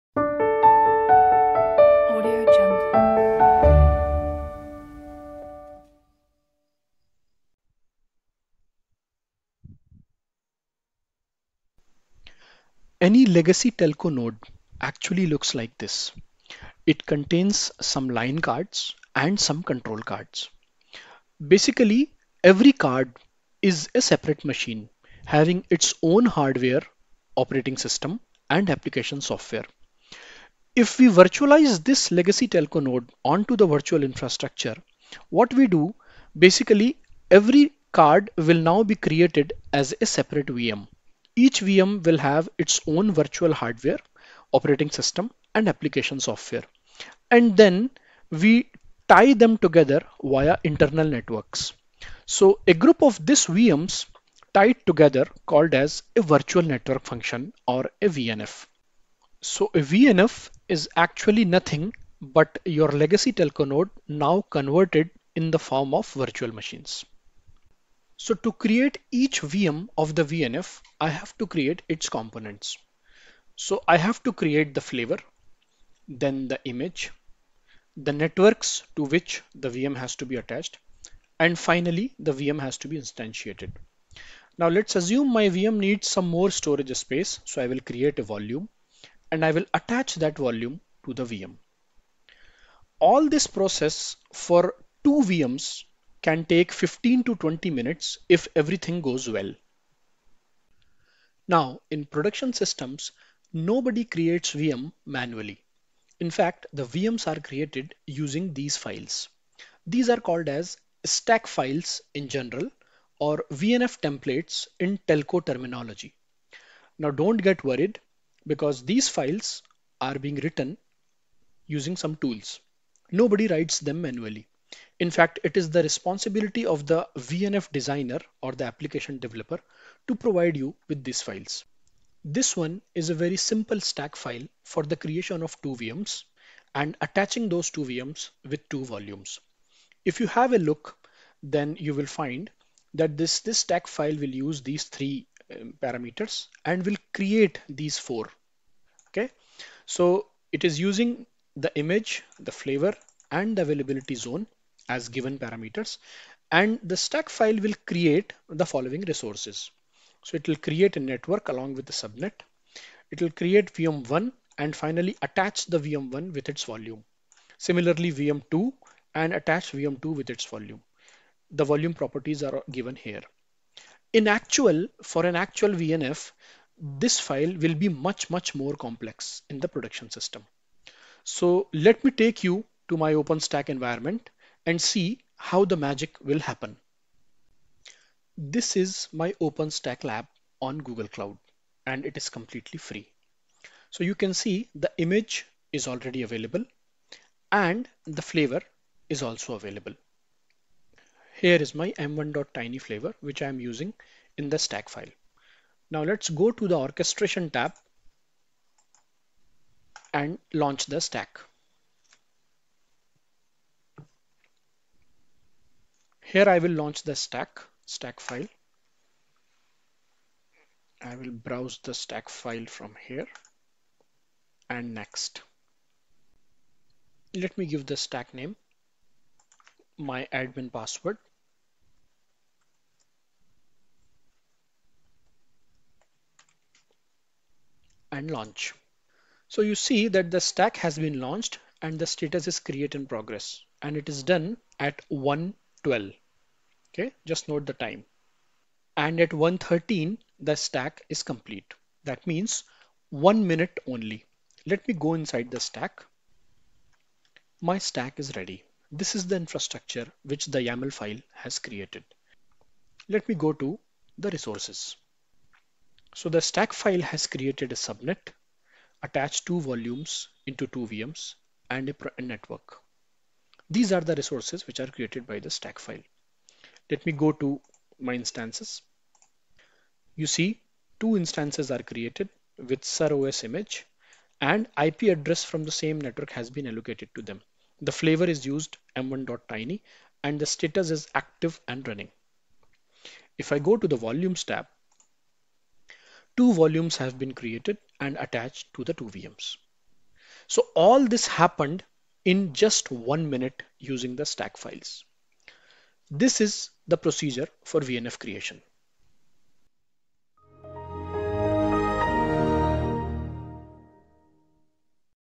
Creative summaries-3 - How to create a VNF in OpenStack
Have you ever wondered how a legacy Telco node like GGSN, HSS, RNC, etc gets converted into a virtual network function (VNF)?
If yes, then this video is just for you.

See the real magic of OpenStack heat engine. You will find the answer to your curiosity till now.

Full course available on Udemy